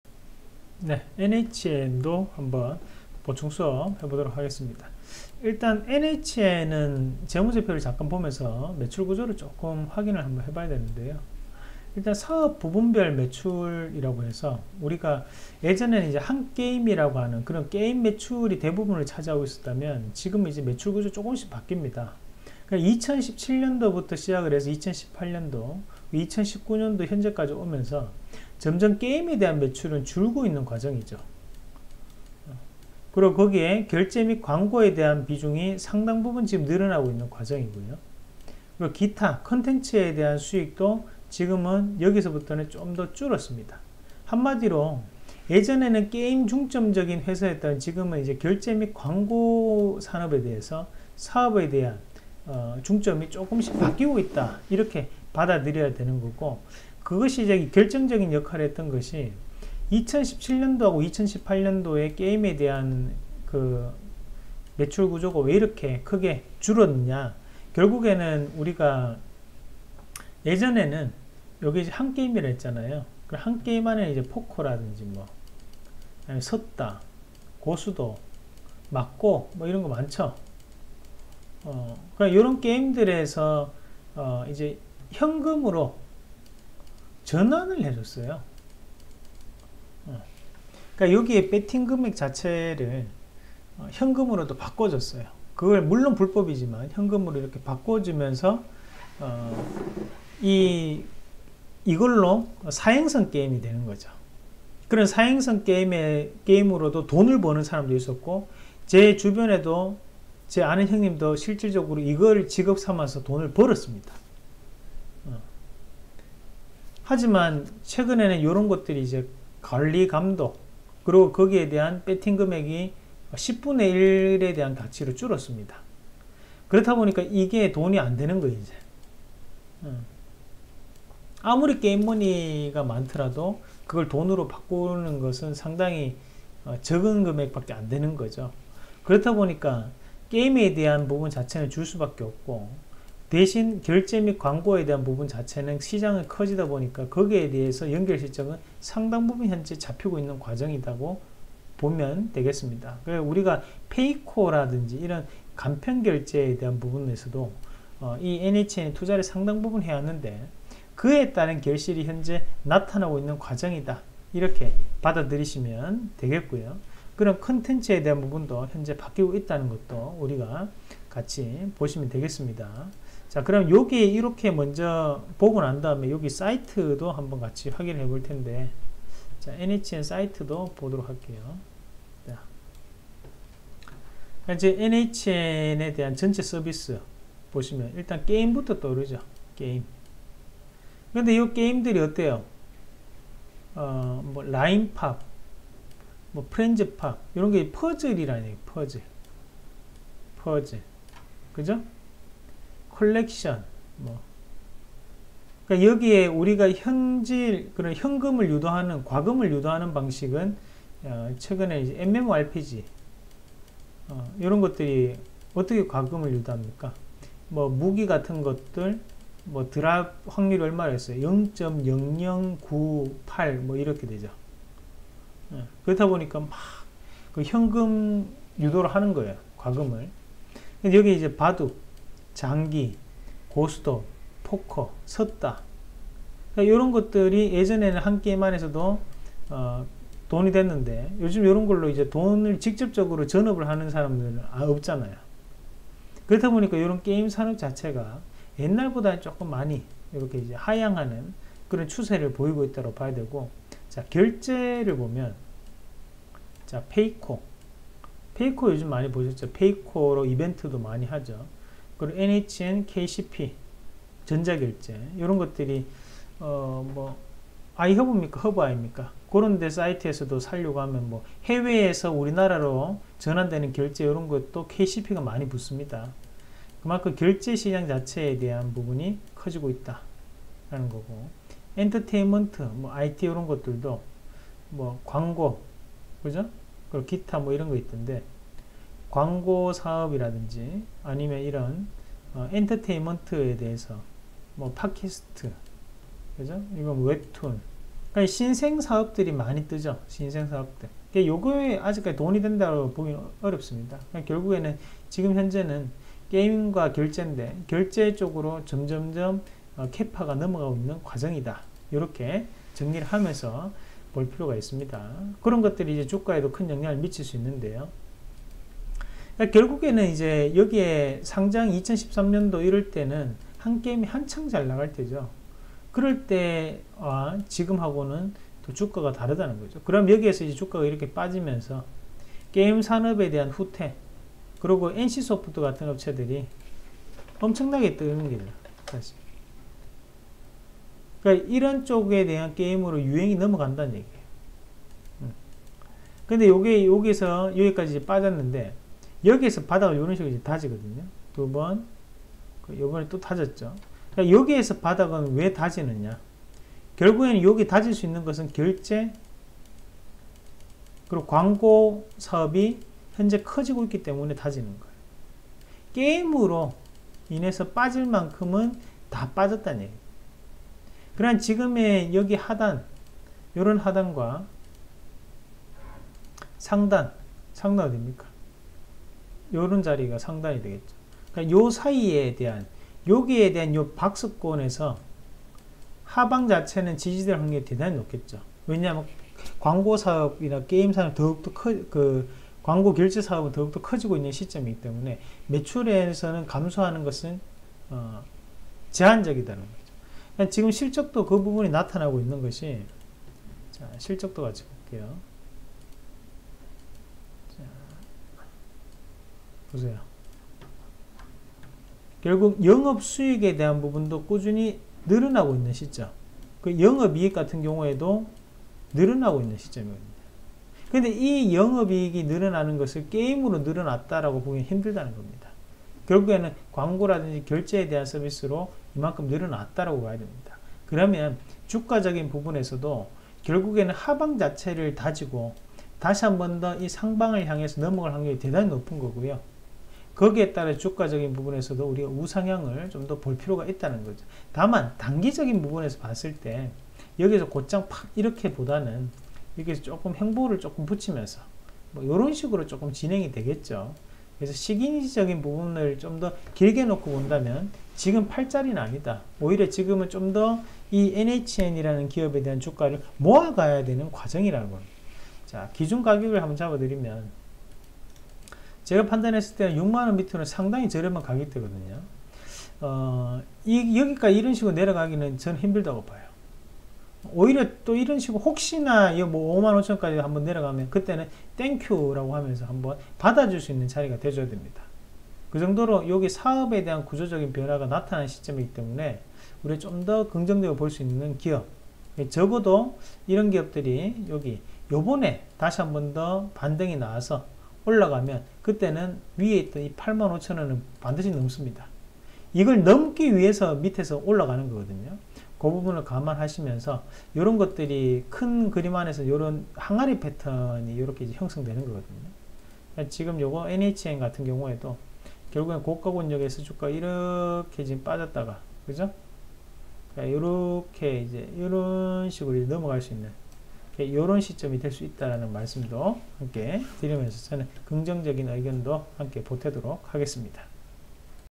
NHN 한게임을 넘어서나? NHN한국사이버결제 업고 이제는 PAYCO 전자결제로? 페이관련주 대응방법, 자사주취득 분석, 이상로의 차트분석 & 차트공부
1. NHN 한게임 매출처다변화 스토리
2. NHN한국사이버결제 연결실적 분석
3. 페이관련주 대응방법